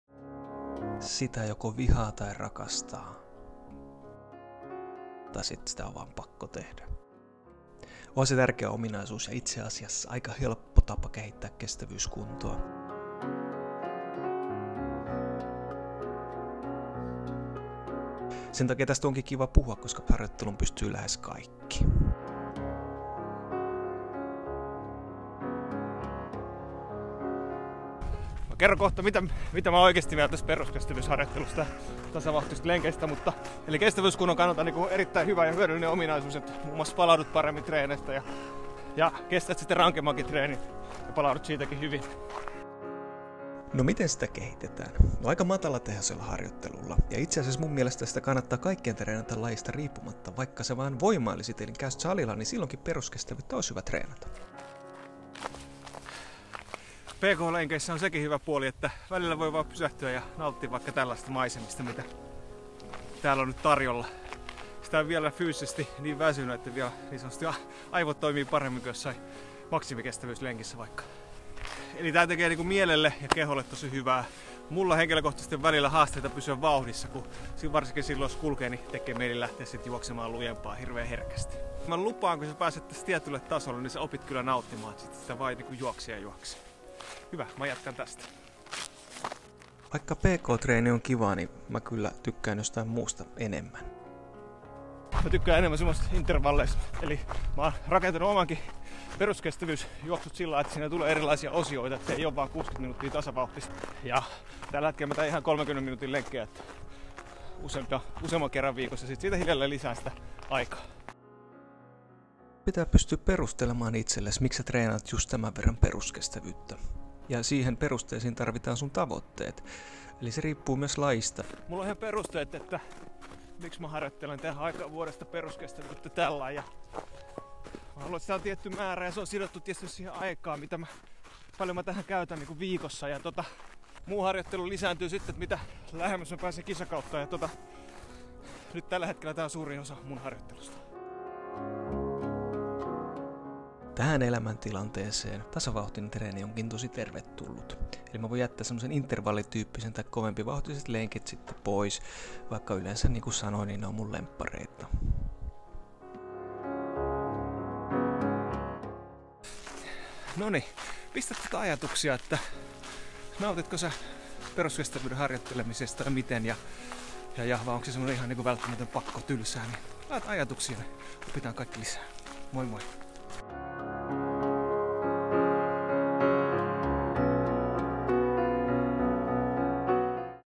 Näin rakennan treenini, osa 5 | Peruskestävyys
Tällä videolla pohdin peruskestävyyden vaikutuksia yleisesti sekä omassa harjoittelussa. Tämä on videosarjan viides osa. Muut osat löydät tältä kanavalta.